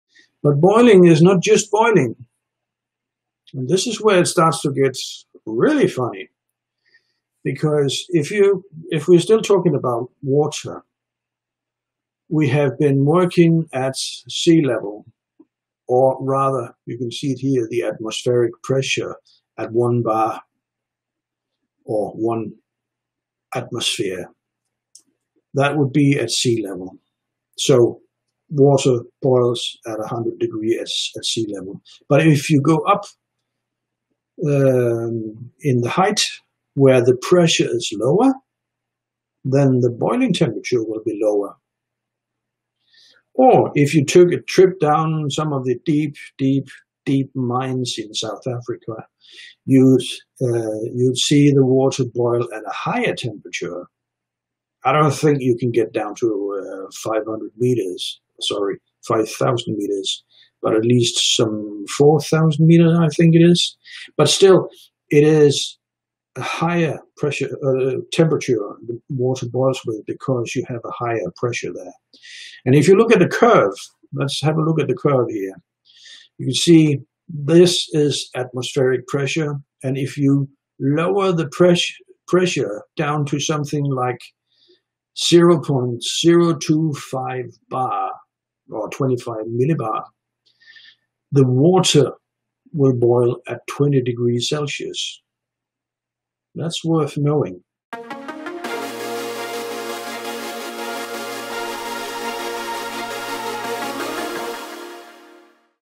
Why does water's boiling point change at different levels of pressure? | Ask an Expert
In this clip from his Refrigeration 101 Q&A Livestream, Jens Andersen answers the question: Why does water's boiling point change at different levels of pressure?

Jens talks about the relationship between water and pressure. He explains how the atmospheric pressure affects the temperature at which water boils. When water is at high altitudes, it boils at a lower temperature than 100 degrees Celsius. When water is beneath sea level, it boils above 100 degrees Celsius.